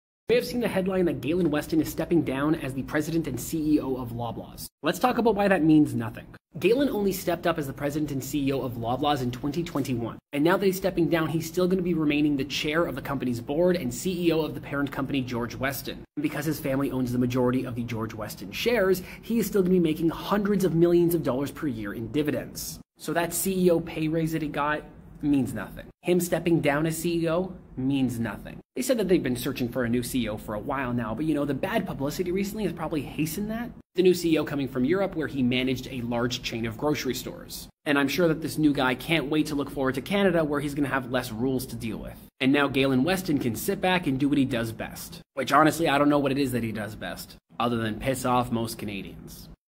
Galen Weston is stepping down as Loblaw president
Galen Weston is stepping down as Loblaw president. Why does this not matter? canada cdnpoli galenweston loblaws loblawsCanada frankdomenic🍁